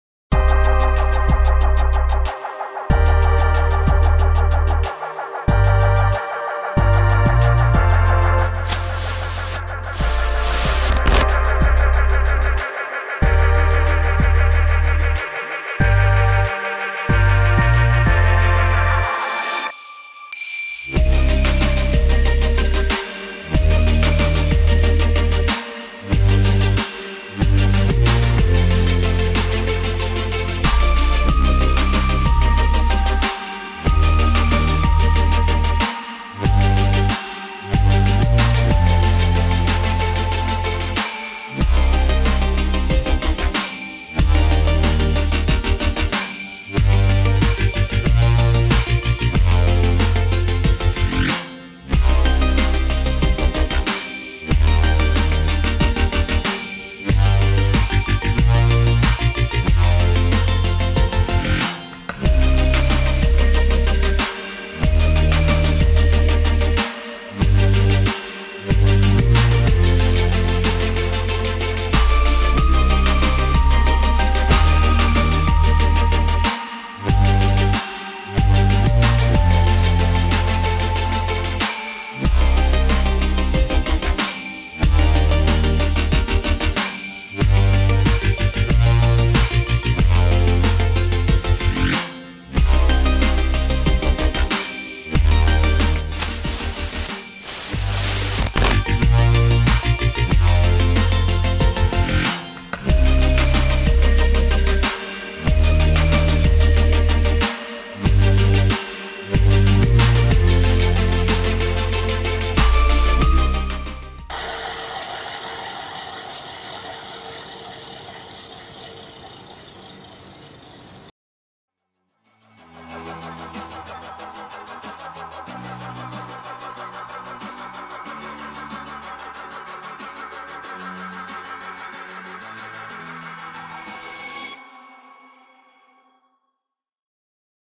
NS Bikes - RAG 2+ 2020 Bike - REAL WEIGHT!! Hub Sound
Gravel bikes have become incredibly popular and we were in the game from the begging with a bike that had dirt heritage in it's DNA. The RAG+ has become a favorite among our employees and friends, even those who would never even consider a drop bar bike before. The range of applications is endless - from commuting to amateur CX racing, or simply as a fitness bike. A properly scaled down version of the Rag is also available on 26” wheels, and is perhaps one of the only really gravel specific kid's bikes on the market.

RAG is short for Road And Gravel and the 'plus' is there because you can run it with standard 700C road tires, 700C gravel tires (up to 45C) or even 27.5” wheels with tires as big as 2.2”! Perfect for anyone who wants a REAL all round drop bar bike. Use it for bike packing, gravel grinding, amateur CX racing or commuting. If you mount 28mm road tires you can hit the tarmac and easily keep up with your roadie buddies. It features the latest technologies including a proper 1X drivetrain with a wide range cassette, disc brakes and thru-axles.